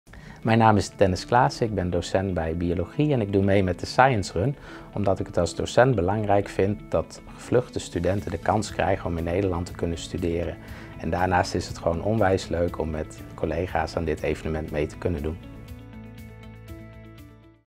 Dennis Claessen - Leiden Science Run 2018
The Leiden Science Run is a 5 km charity run at the Bio Science Park Leiden. It is a relay race organized by Leiden University in collaboration with the University Sports Centre and running club Currimus.

This charity run will be in the form of a 5km relay race, with teams of four people (each team member runs 5 km). The goal is to raise as much money as possible for the UAF (Foundation for Refugee Students). In addition to the registration fee of € 20, - we ask the participants to engage family, friends and colleagues to sponsor them.